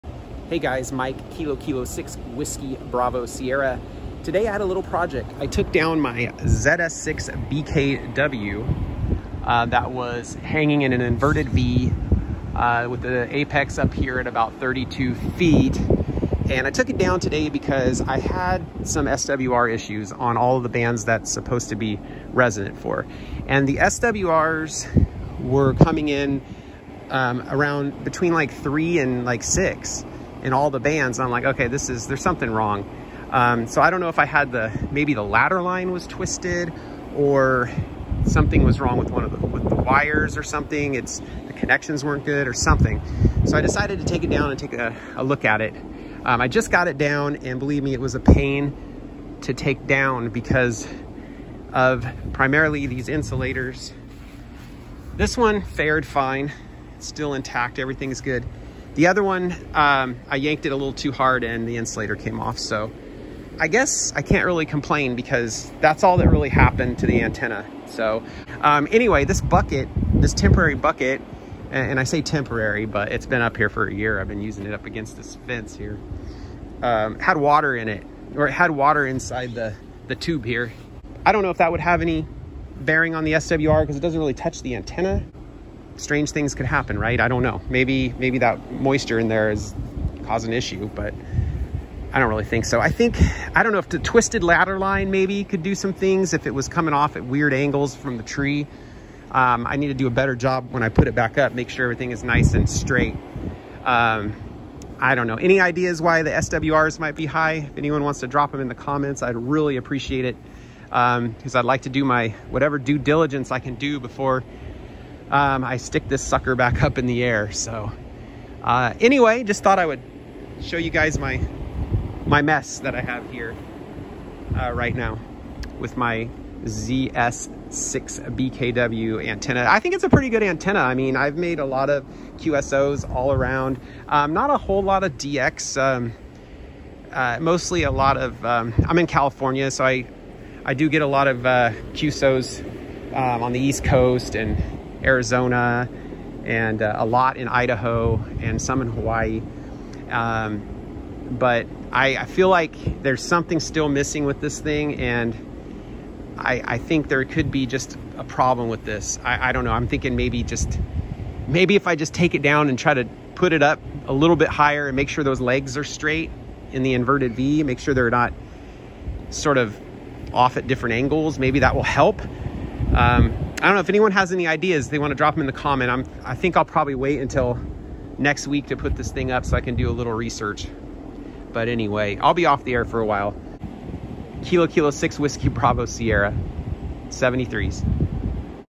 ZS6BKW SWR Issues
Had to take down my ZS6BKW to inspect it and figure out why the SWR’s are high on all bands.